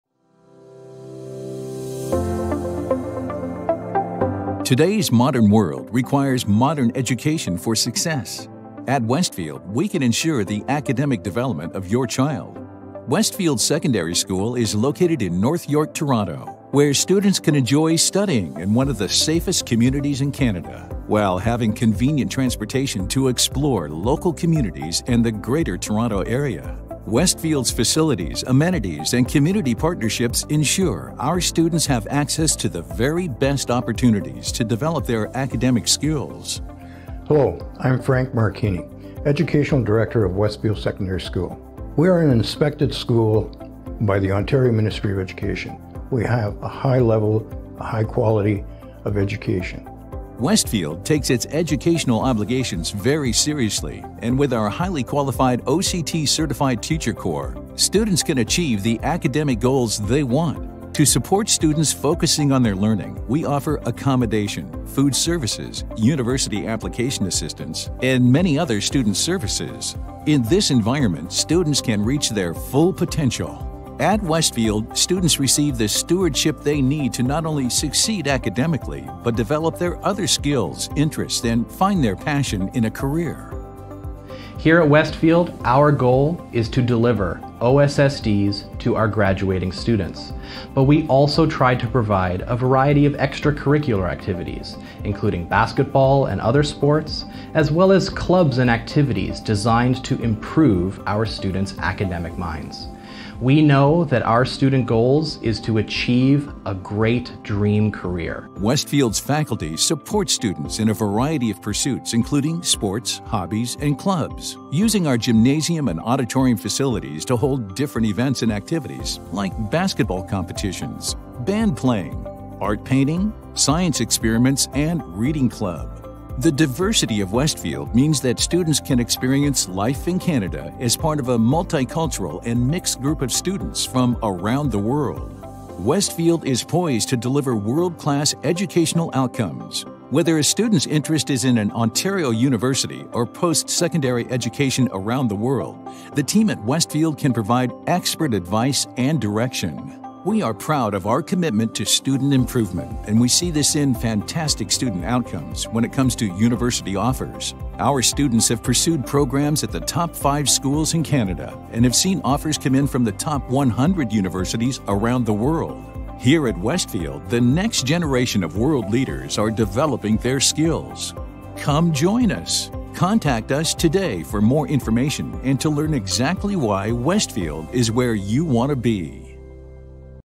Westfield Secondary School - New Campus
Westfield Secondary School has been relocated to 33 Mallard Rd, North York. Our new campus is boasting a 3-story building with lots of amenities.